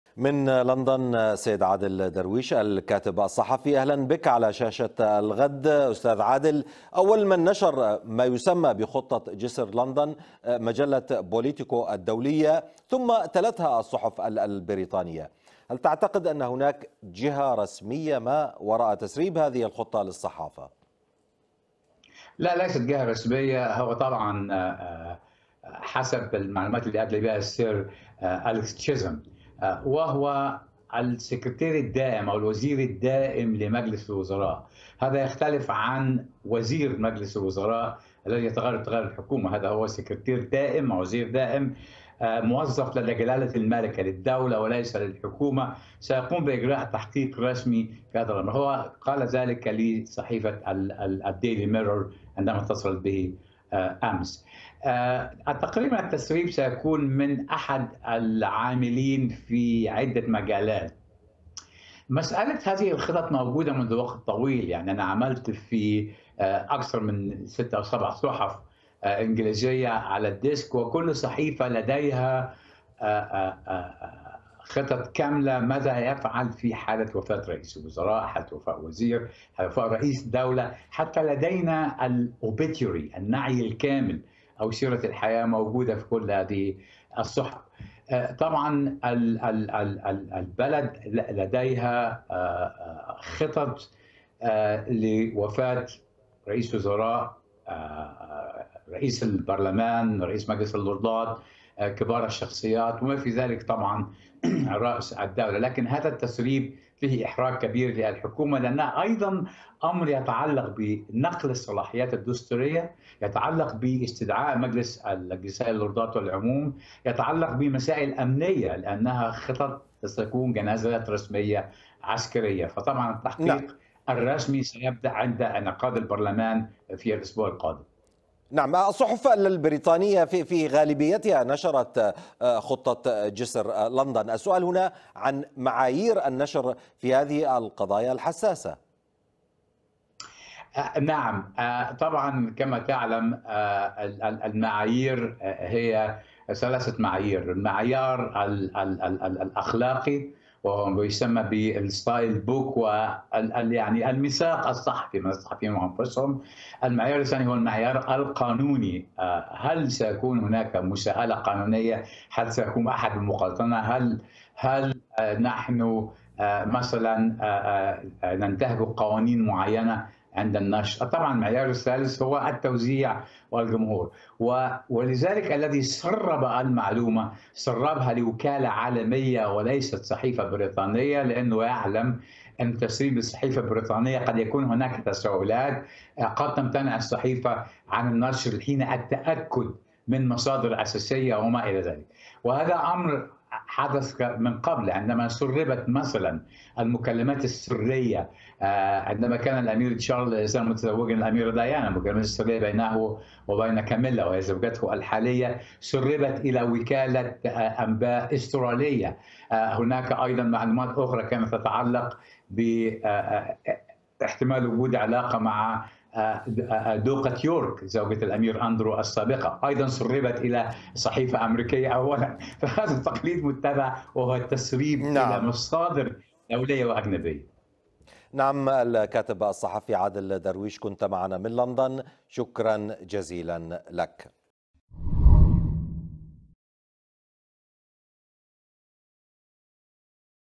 عادل درويش: تسريب وثائق التعامل مع وفاة الملكة هو إحراج للحكومة البريطانية
فتحت الحكومة_البريطانية تحقيقا بعد تسريب خطط رسمية أطلق عليها اسم جسر_لندن، تضم جميع التفاصيل الرسمية التي تتبع في حال وفاة_الملكة_إليزابيث الثانية والساعات التي ستلي هذا الإعلان والحذر من تدفق السائحين المتوقع عند الإعلان عن وفاة الملكة.
المزيد من التفاصيل مع عادل درويش الكاتب الصحفي.